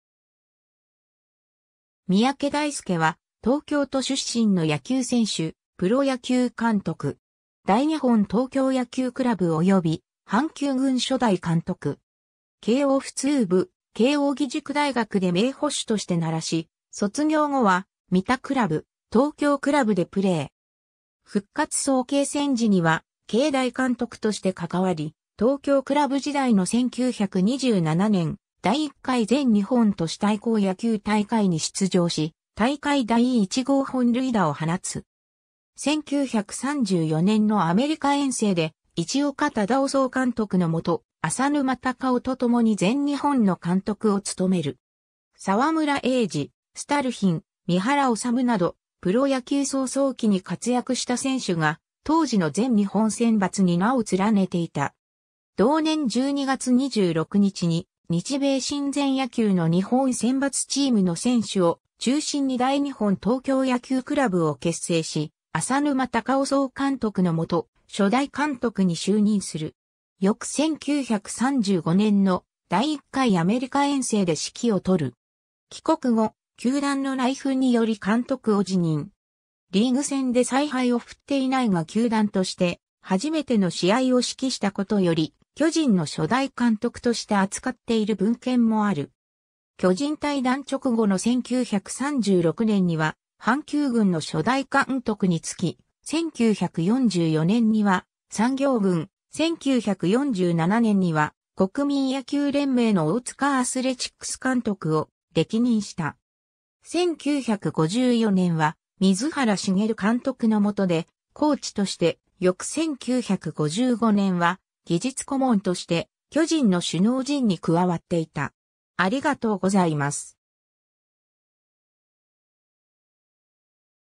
三宅大輔
オリックス・バファローズ及びその前身球団の監督
三宅 大輔（みやけ だいすけ、1893年4月16日 - 1978年1月3日）は、東京都出身の野球選手・プロ野球監督。
大日本東京野球倶楽部及び阪急軍初代監督。
 慶應普通部、慶應義塾大学で名捕手として鳴らし、卒業後は三田クラブ、東京倶楽部でプレー。
復活早慶戦時には慶大監督として関わり、東京倶楽部時代の1927年、第1回全日本都市対抗野球大会に出場し大会第1号本塁打を放つ。
 1934年のアメリカ遠征で、市岡忠男総監督の下、浅沼誉夫とともに全日本の監督を務める。
沢村栄治、スタルヒン、三原脩などプロ野球草創期に活躍した選手が当時の全日本選抜に名を連ねていた。
 同年12月26日に日米親善野球の日本選抜チームの選手を中心に大日本東京野球倶楽部を結成し、浅沼誉夫総監督の下、初代監督に就任する。
翌1935年の第1回アメリカ遠征で指揮を執る。
帰国後、球団の内紛により監督を辞任。
リーグ戦で采配を振っていないが球団として初めての試合を指揮したことより巨人の初代監督として扱っている文献もある。
 巨人退団直後の1936年には阪急軍の初代監督に就き、1944年には産業軍、1947年には国民野球連盟の大塚アスレチックス監督を歴任した。
 1954年は水原茂監督の下でコーチとして、翌1955年は技術顧問として巨人の首脳陣に加わっていた。